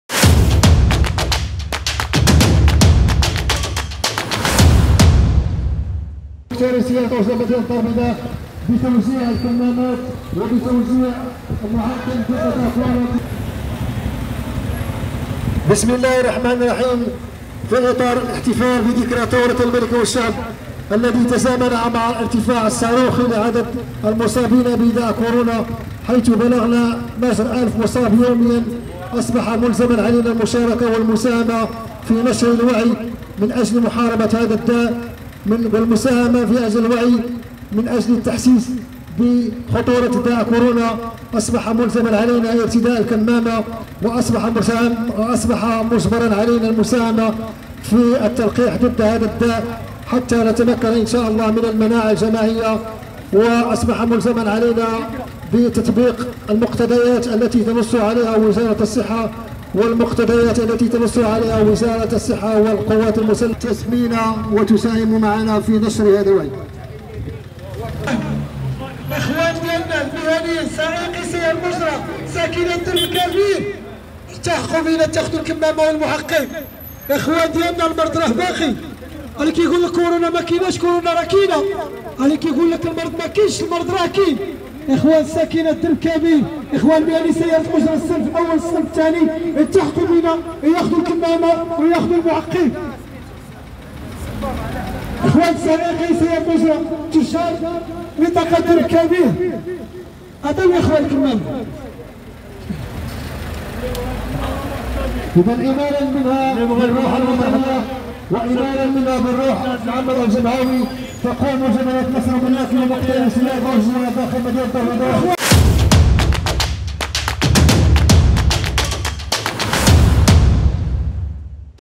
أصحاب سيارات الأجرة يحتفلون بذكرى ثورة الملك والشعب
في ظل التدابير والإجراءات الاحترازية، خلد أصحاب سيارات الأجرة بالدار البيضاء ذكرى ثورة الملك والشعب، مجددين أواصر البيعة

# أخبار_عاجلة